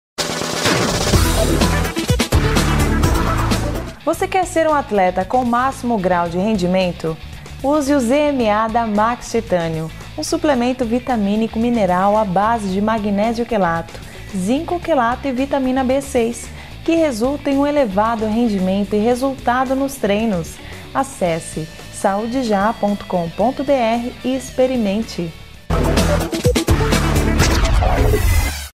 ZMA - Max Titanium
Você quer ser um atleta com o máximo grau de rendimento? Use O ZMA da Titanium, um Suplemento Vitamínico-Mineral a base de magnésio quelato, zinco quelato e vitamina B6 que resulta em um elevado rendimento e resultado nos treinos. Acesse e experimente!